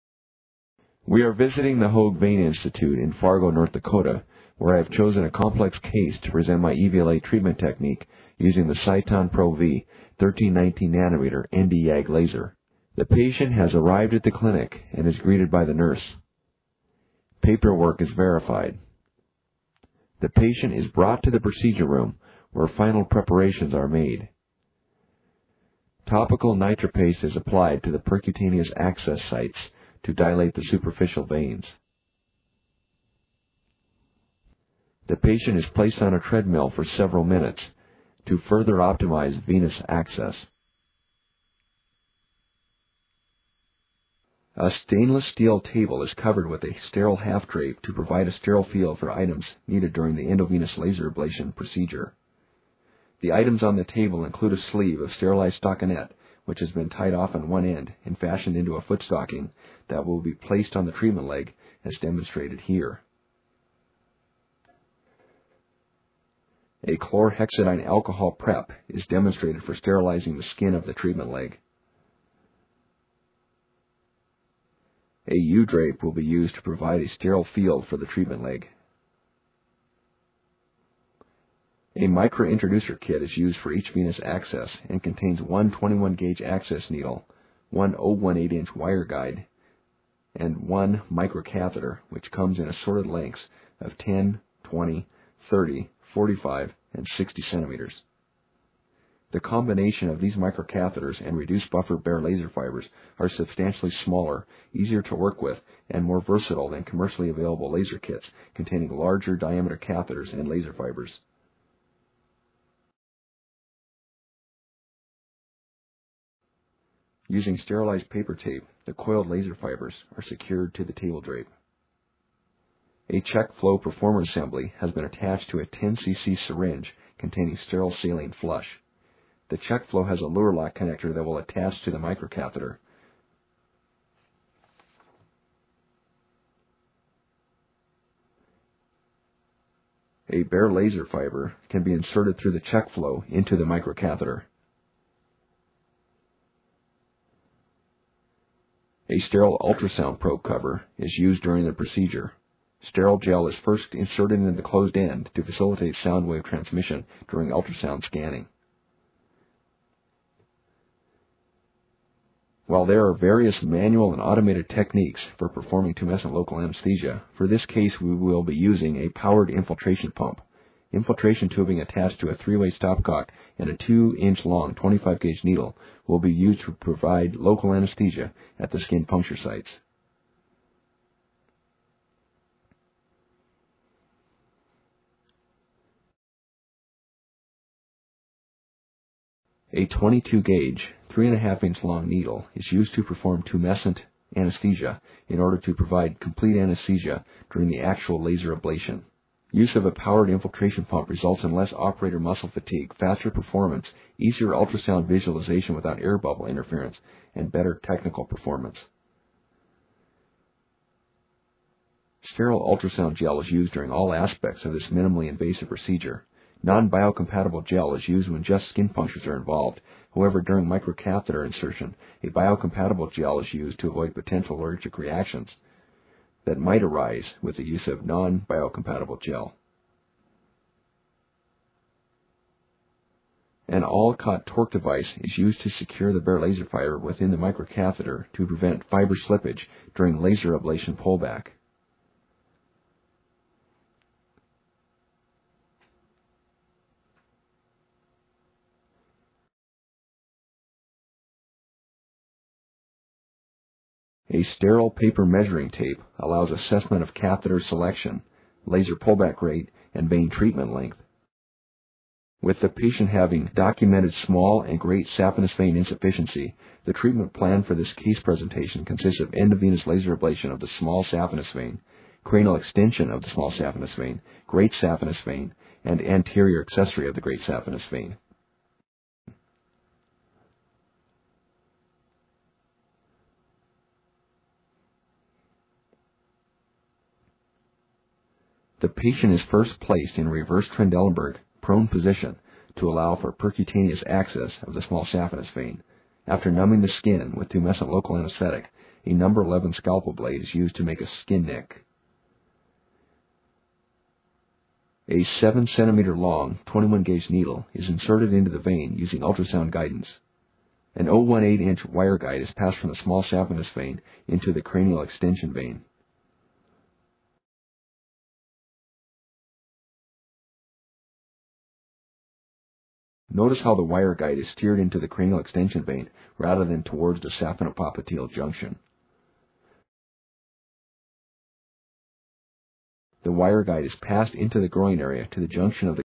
Endovenous Laser Ablation (EVLA) Treatment Procedure
EVLA procedure performed at HogueVeinInstitute in Fargo, ND